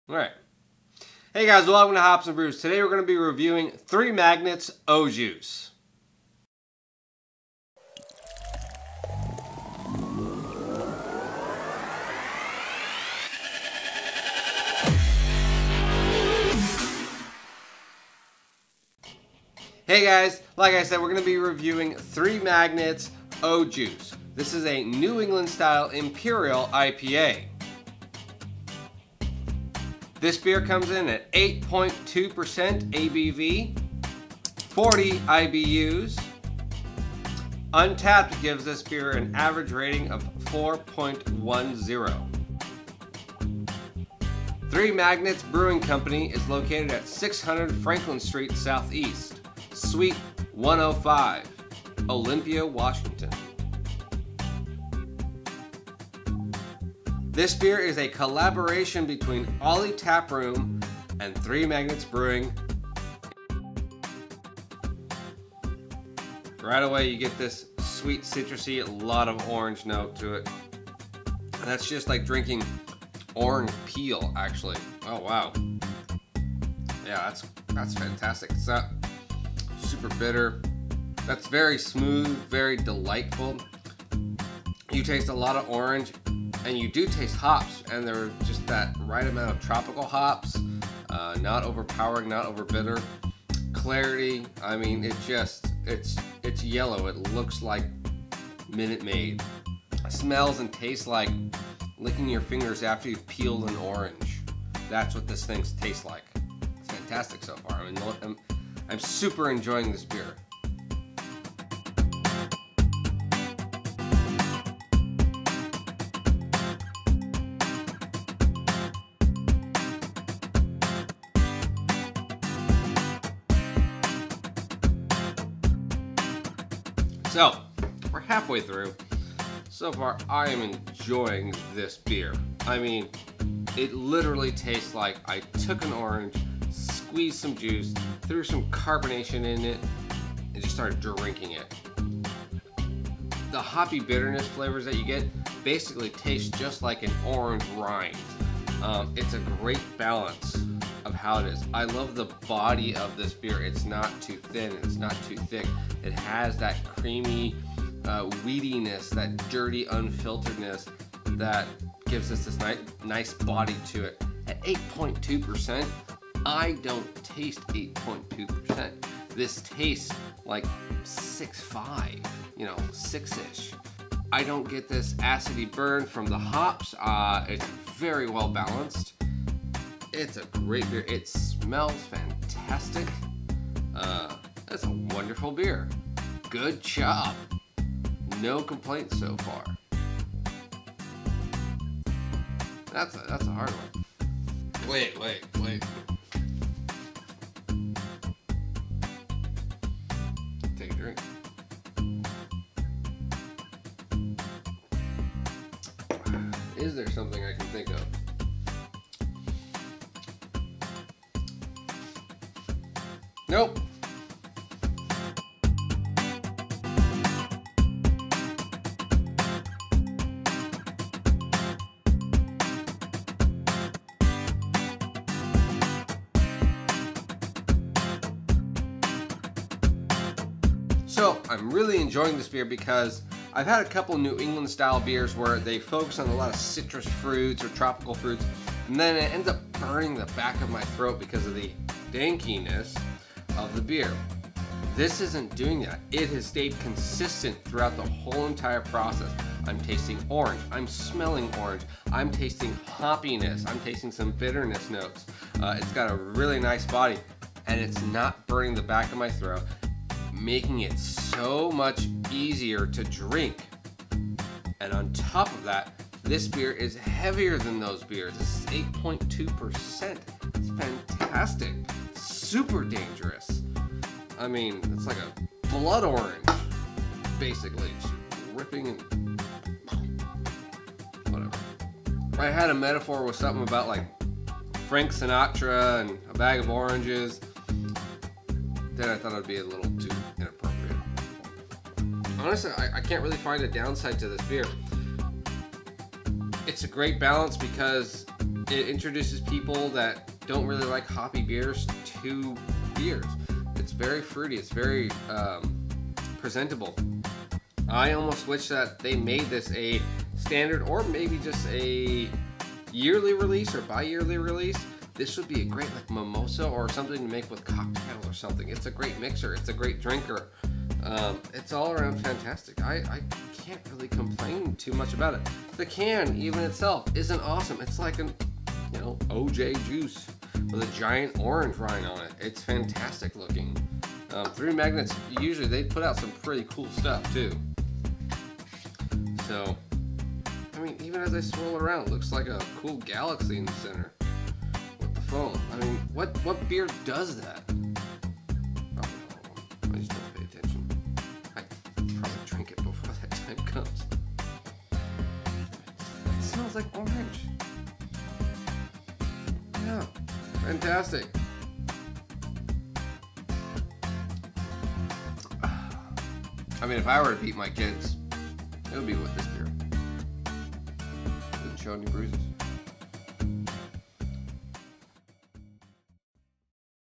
Three Magnets - OJuice BEER REVIEW
BEER NAME: Three Magnets Brewing - OJuice Imperial New England IPA
ABV: 8.2%
IBU: 40

UNTAPPD: 4.10/5

Profile:" Brewed in celebration of Oly Taproom's 3rd Anniversary, our latest in the Juice series features a soft bitterness and packs a huge dry hop component of Simcoe, Cascade, Citra and Amarillo hops. With  a thick orange hue in the glass, there's sure to be debate in whether you're drinking a beer or orange juice!"